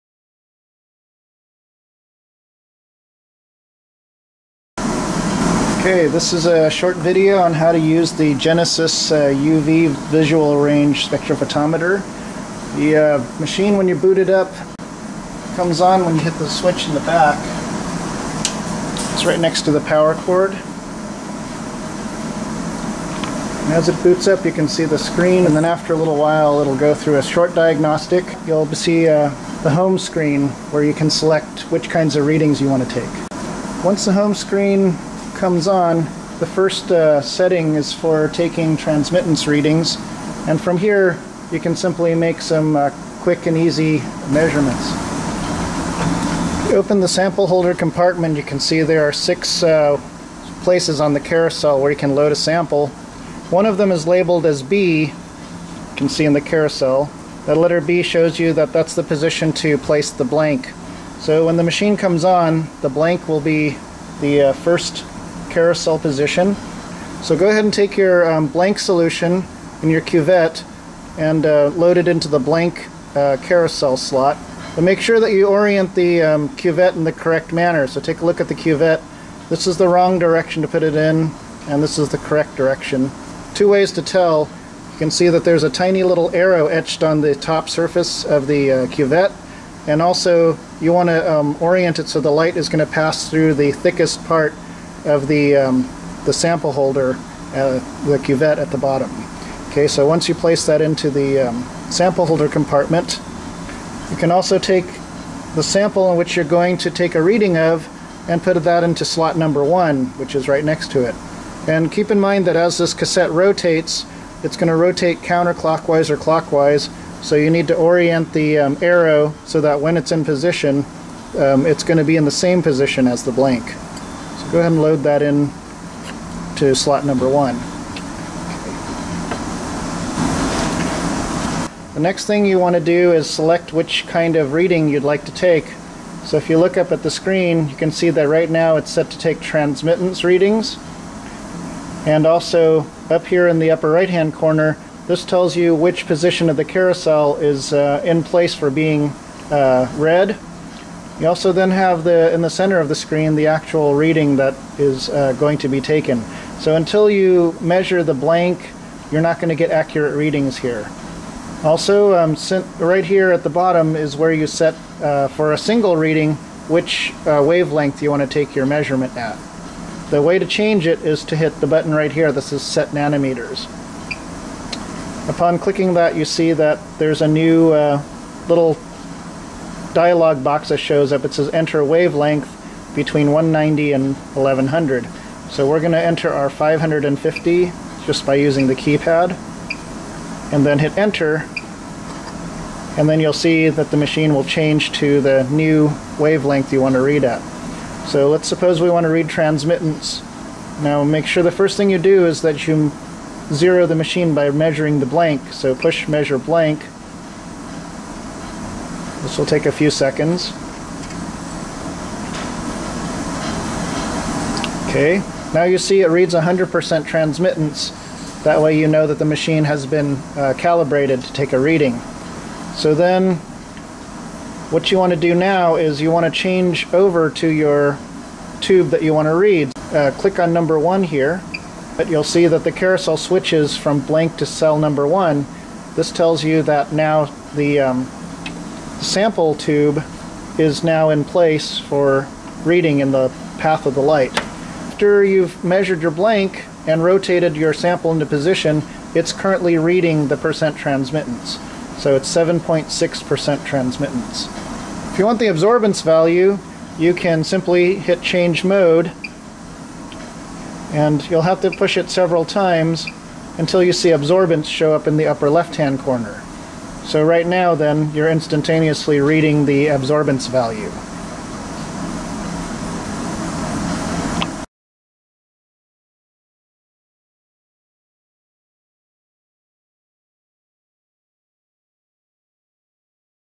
How to Use the Genesys UV-Vis Spectrophotometer (Part 1)
Part 1 of 2.  How to take a single %T or A reading at a single wavelength.  Part 2 continues with how to obtain an Absorbance spectrum profile.